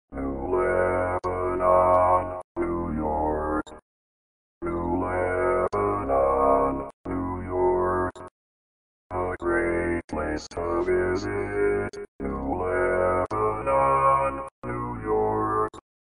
How to Say or Pronounce USA Cities — New Lebanon, New York
This video shows you how to say or pronounce New Lebanon, New York.

A computer said New Lebanon, New York. How would you say New Lebanon, New York?